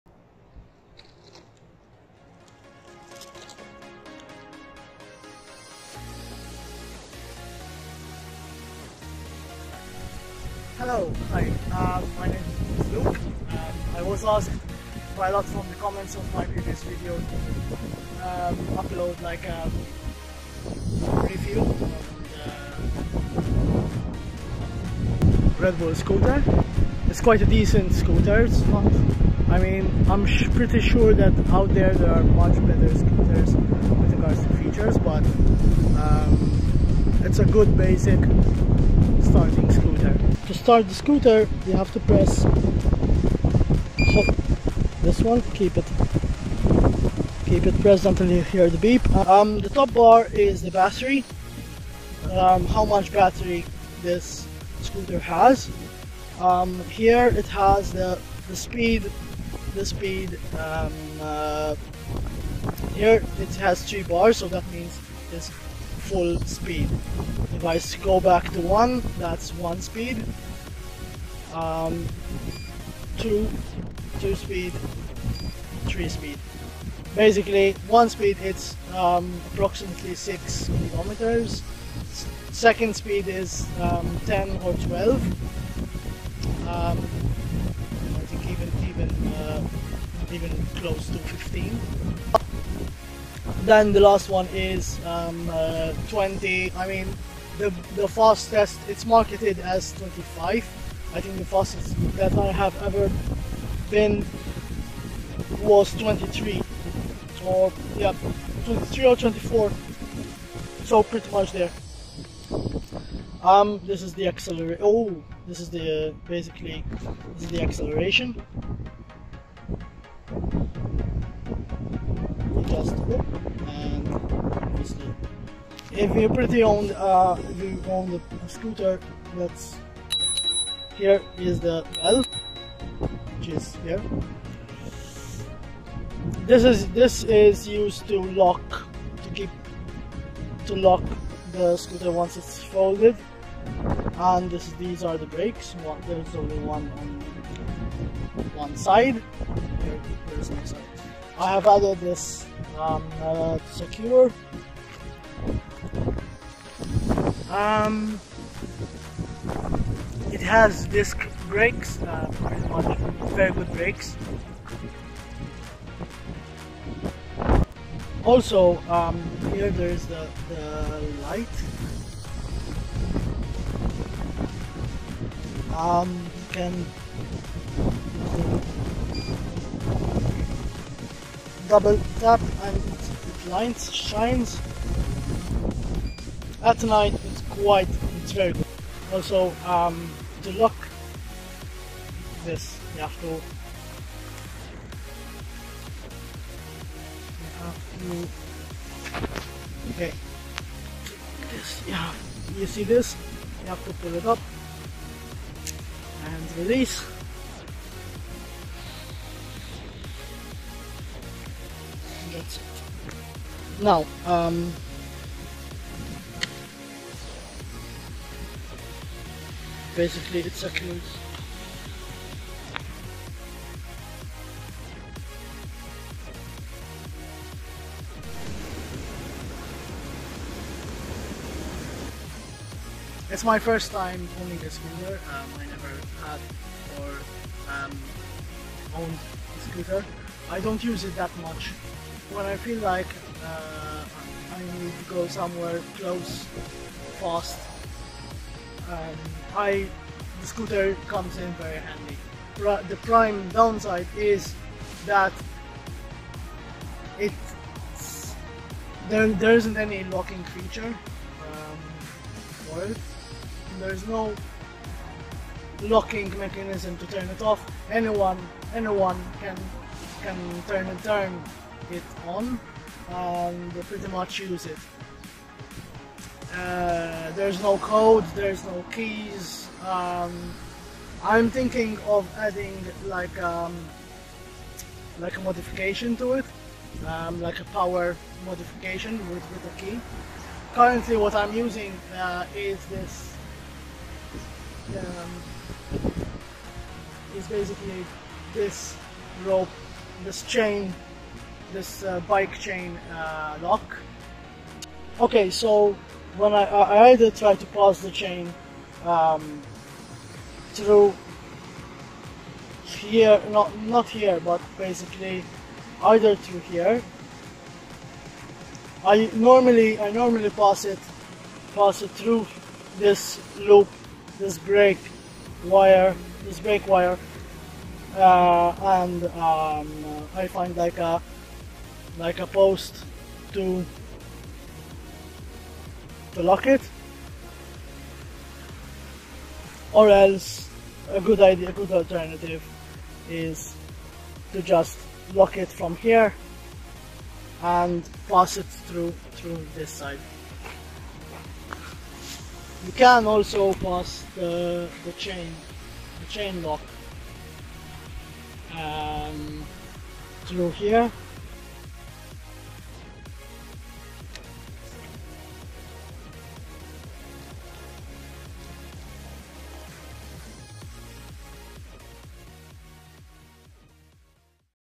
Red Bull Racing 8.5" Race Teen Foldable E-scooter [Overview]
Join me in my latest video as I take a closer look at the Red Bull Racing 8.5" Race Teen Foldable E-scooter. This sleek and convenient scooter offers a perfect blend of style and functionality. I'll walk you through its cool foldable design, its eye-catching race-inspired aesthetics, and its practical features. Whether you're a teen looking for a fun and efficient mode of transportation or simply a fan of Red Bull Racing, this video review will give you all the details you need to know about this awesome scooter. Get ready for an enjoyable and informative ride!